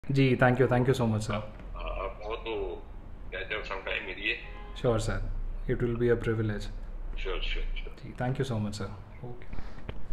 MOS in PMO Dr. Jitendra Singh wishing Civil Service Qualifier from Bhaderwah
MOS in PMO Dr. Jitendra Singh wishing Civil Service Qualifier from Bhaderwah Dr. Asrar Ahmed.